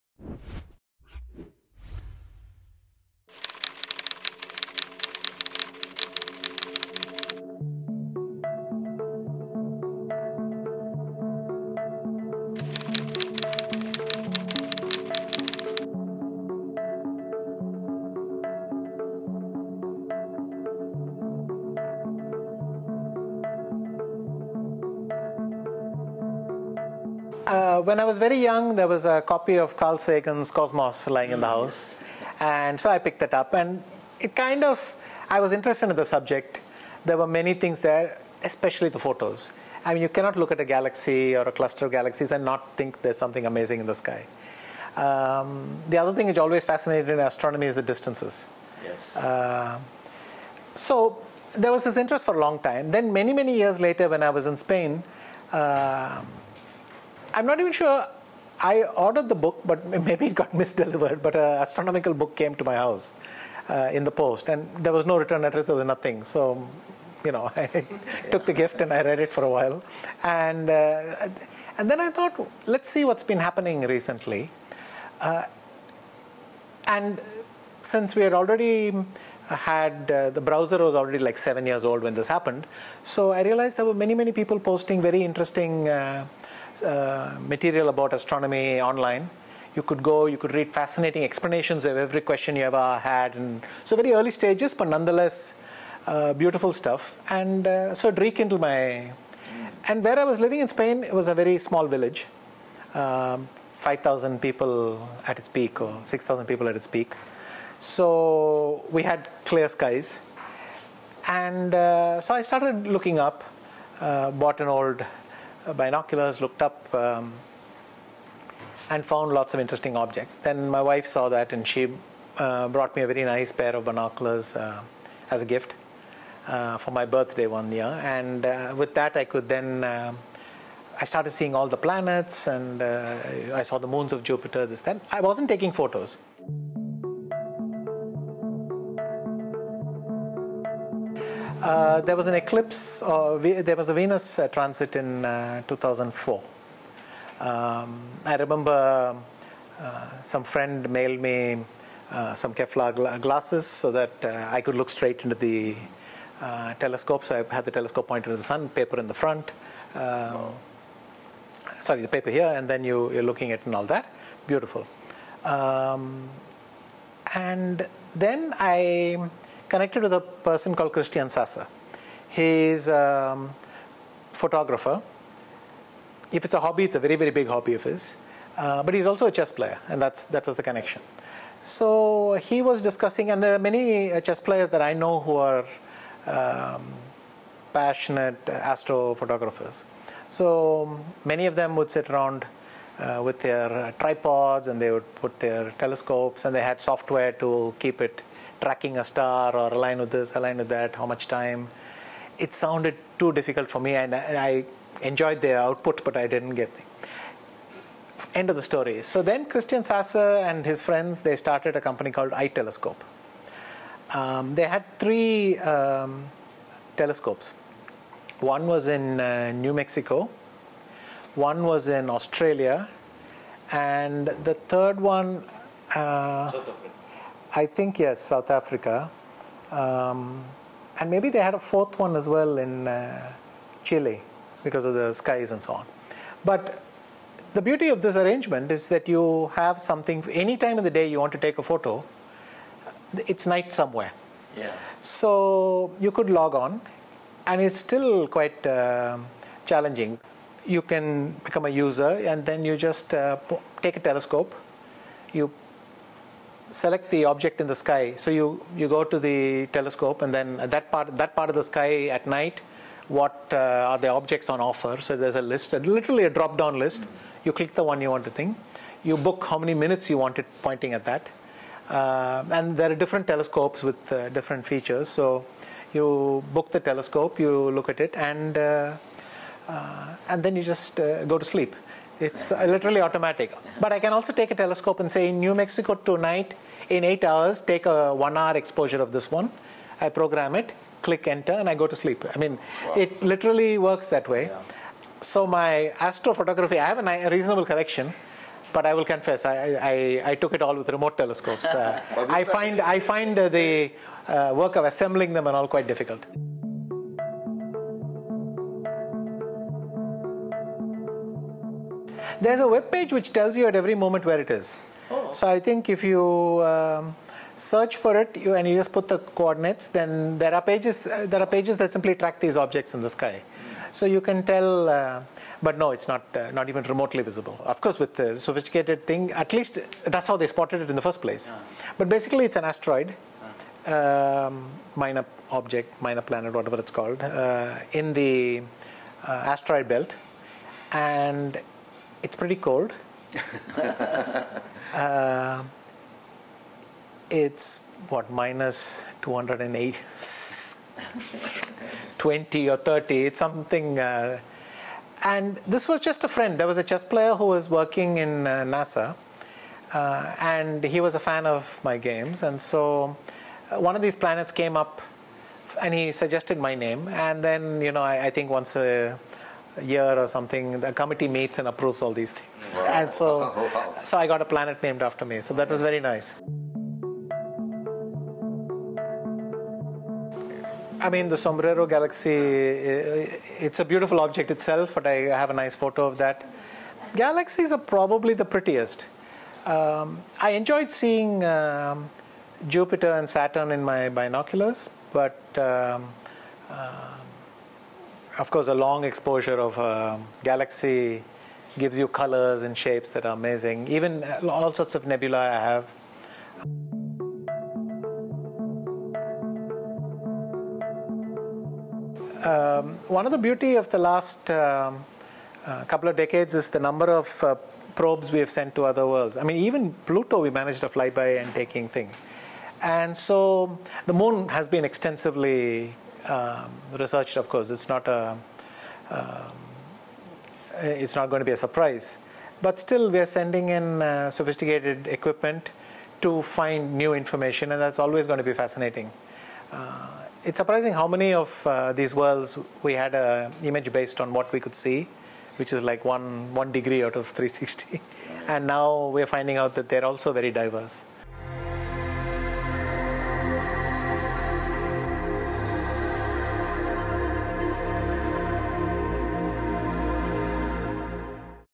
Exclusive interview | Viswanathan Anand on his collection of astrophotography | THE WEEK
Viswanathan Anand has been champion of the world five times. Perhaps that is why one of his hobbies is to look out beyond earth, into space. He is an astronomy enthusiast, an astro-photographer and also has a minor planet named after him. In an exclusive interview with THE WEEK, Anand opens up about this lesser-known aspect of his life.

Coimbatore- 2241911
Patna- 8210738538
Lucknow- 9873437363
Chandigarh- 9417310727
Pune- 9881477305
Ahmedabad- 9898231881